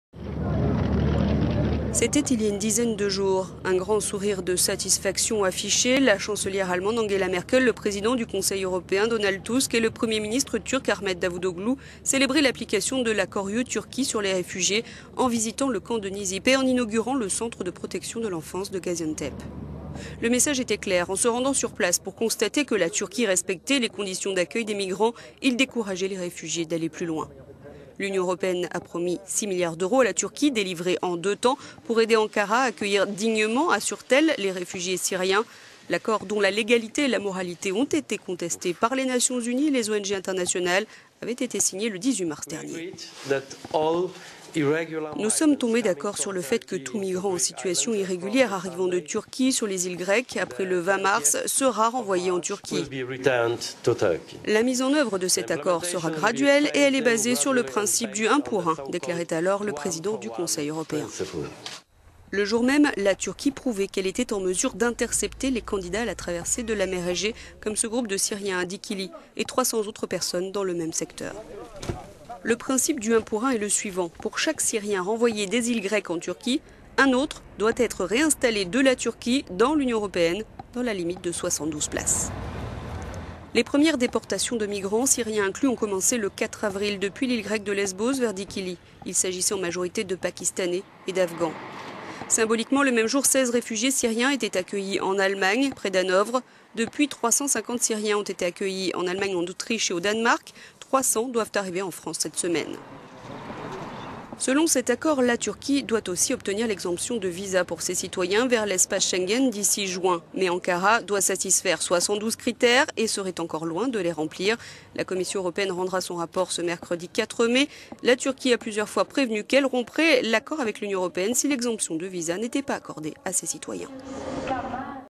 Réfugiés : l'accord controversé entre l'UE et la Turquie fonctionne, mais jusqu'à quand ?
C'était il y a une dizaine de jours, un grand sourire de satisfaction affiché,  la chancelière allemande Angela Merkel, le président du Conseil européen Donald Tusk, et le Premier ministre turc Ahmet Davutoglu  célébraient l'application de l'accord UE-Turquie sur les réfugiés en visitant le camp de Nizip et en inaugurant le centre de protection de l'enfance de Gaziantep...

Le message était clair : en se rendant sur place pour constater que la Turquie respectait les conditions d'accueil des migr…